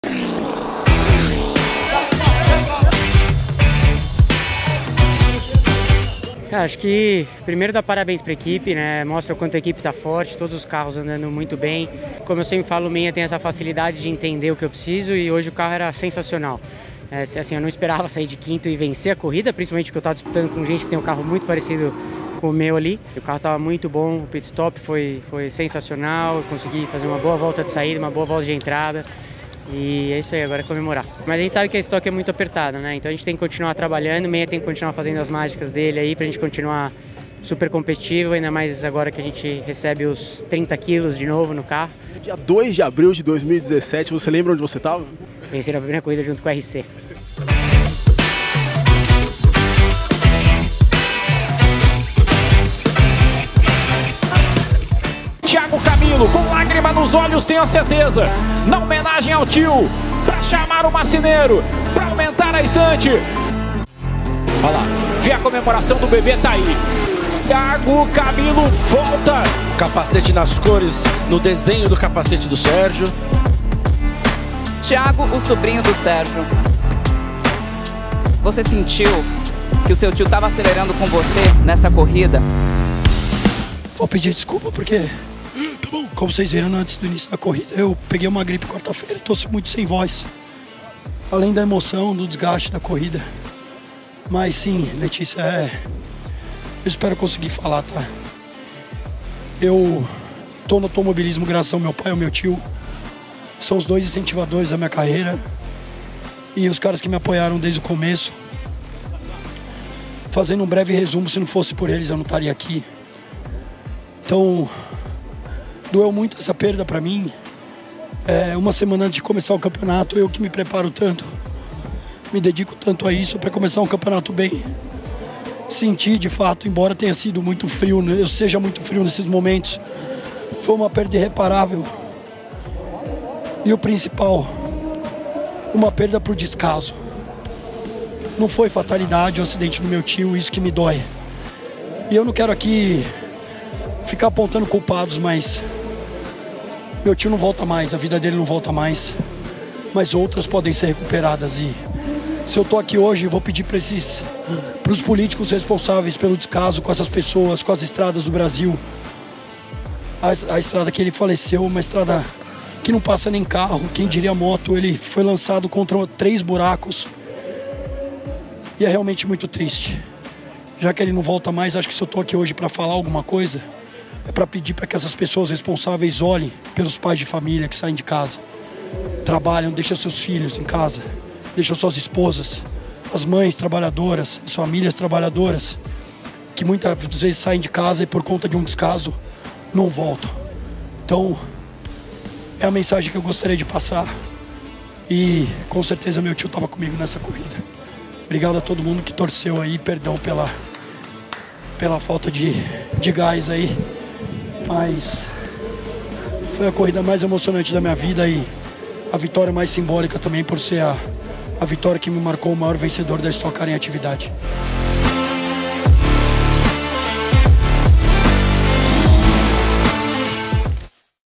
STOCK CAR 2023 | DANIEL SERRA E THIAGO CAMILO VENCEM NA ABERTURA DA TEMPORADA EM GOIÂNIA
A abertura da temporada 2023 da Stock Car Pro Series teve duas corridas emocionantes no Autódromo Ayrton Senna, em Goiânia. Daniel Serra e Thiago Camilo venceram as duas provas, com direito a muita emoção após o pódio.